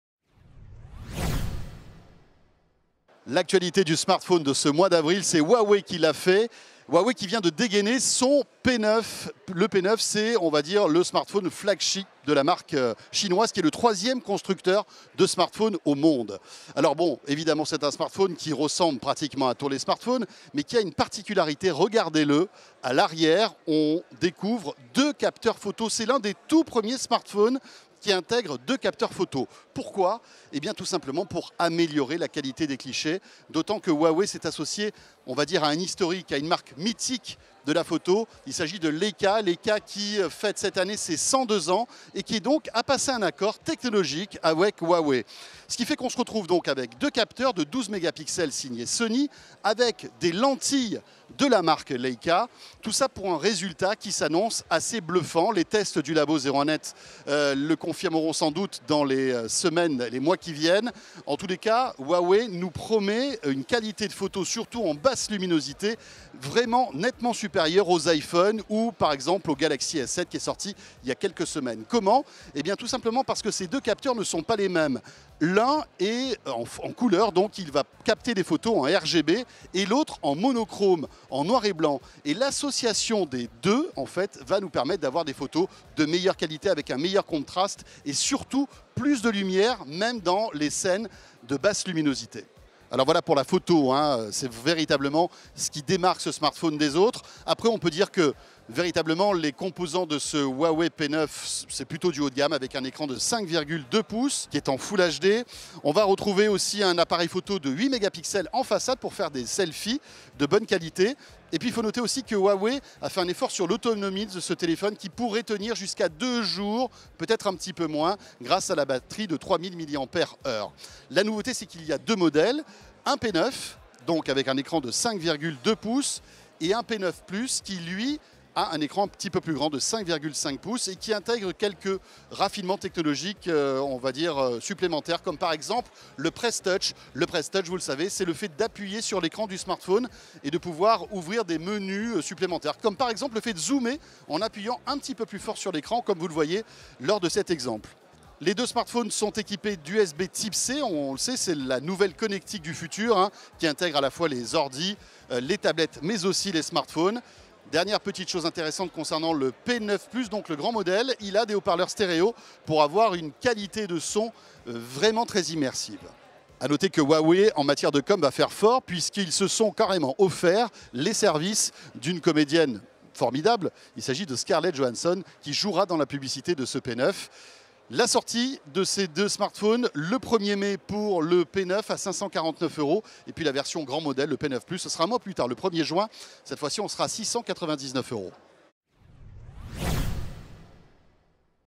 Huawei P9 : Un double capteur photo conçu avec Leica
Huawei a choisi Londres cette année pour présenter à la presse son nouveau smartphone haut de gamme, le P9. le constructeur chinois innove avec ce modèle en intégrant un double capteur photo au dos du téléphone, élaboré en étroite collaboration avec la prestigieuse marque Leica. En combinant une double prise de vue (l'une en couleurs et l'autre en noir et blanc) Huawei nous assure pouvoir réussir des clichés en basse luminosité jusque là impossibles à réaliser avec un smartphone standard. Les P9 (écran 5,2 pouces) et P9 Plus (écran 5,5 pouces) sortiront en France respectivement en mai et juin. Comptez 549 euros pour le P9 et 699 euros pour le P9 Plus.